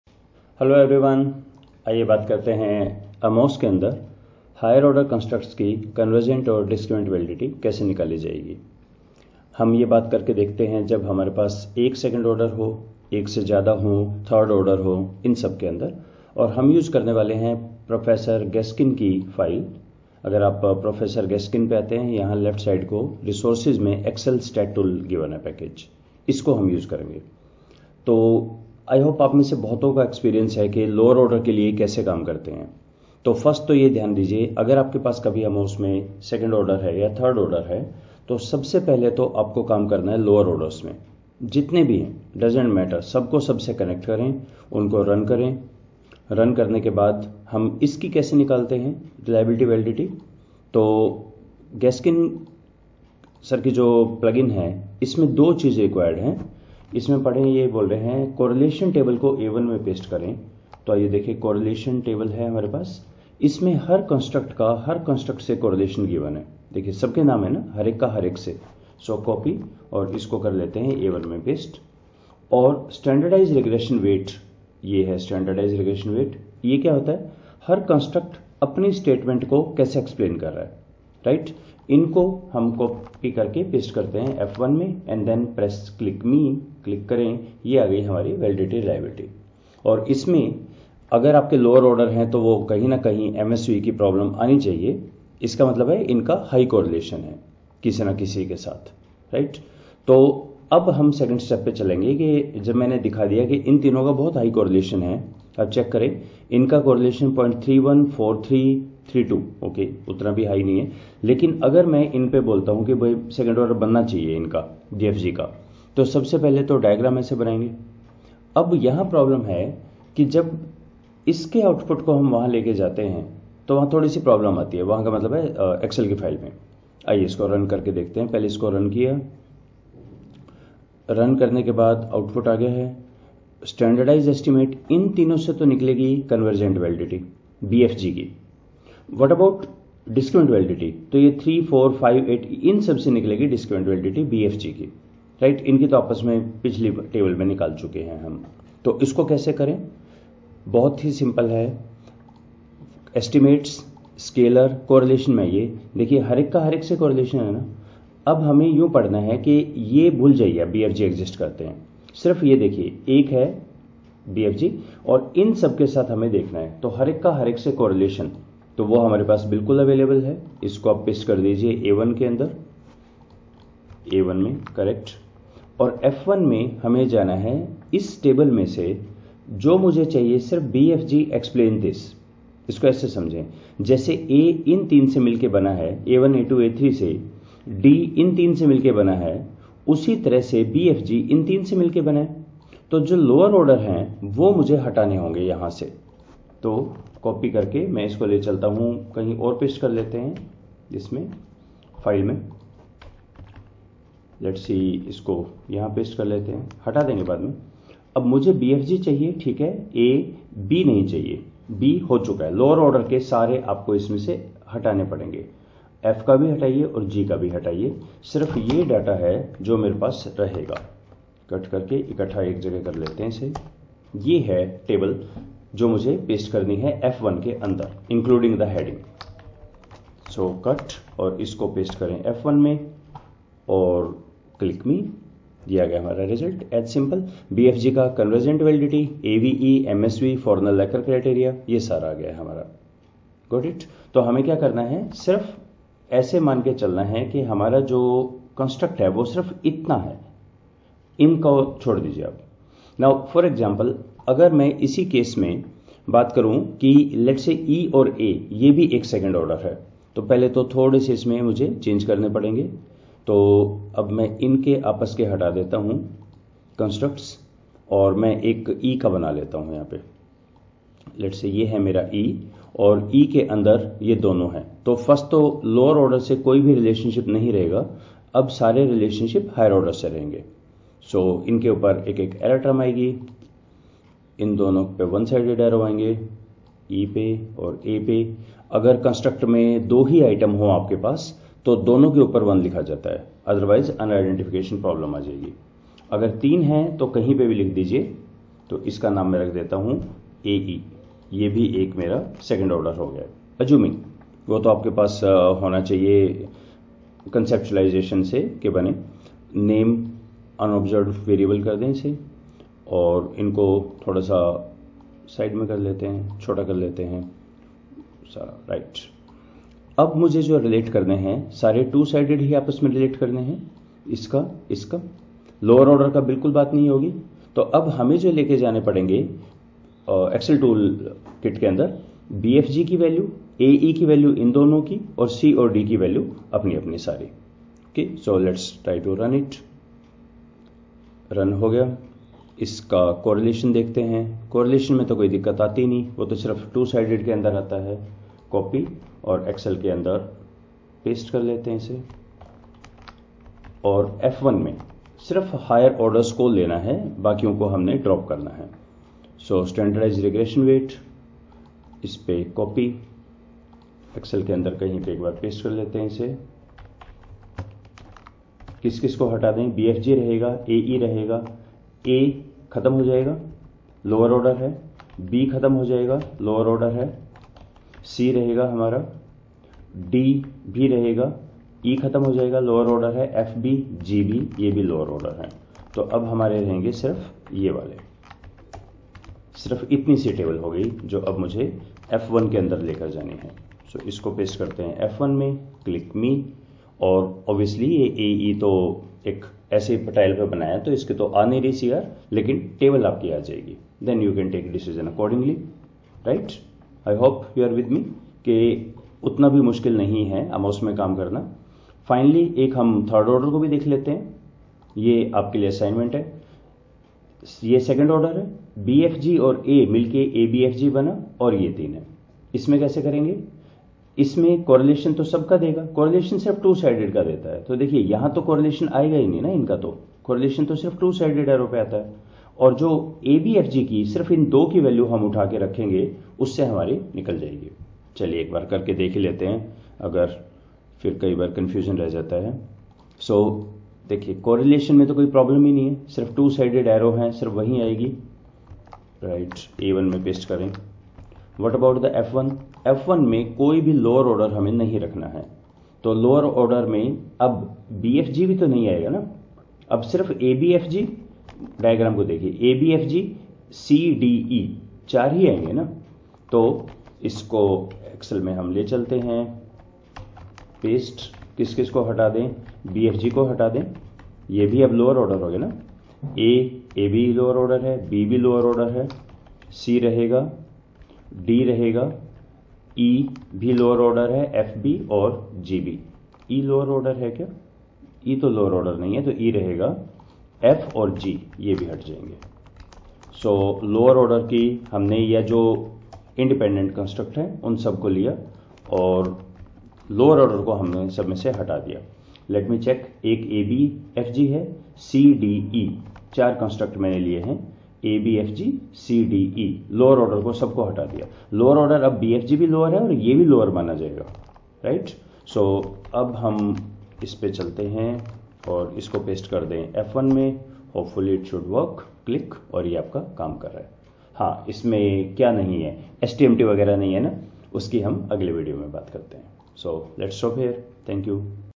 AMOS-28. Convergent and Discriminant Validity of Higher Order Construct using Gaskin's StatTool file
This video discusses the computing of COnvergent and Discriminant Validity of the Higher-order constructs using Prof Gaskin's STAT TOOL Package file.

Datafiles used in this video:
AMOS Second-order1 (All lower orders)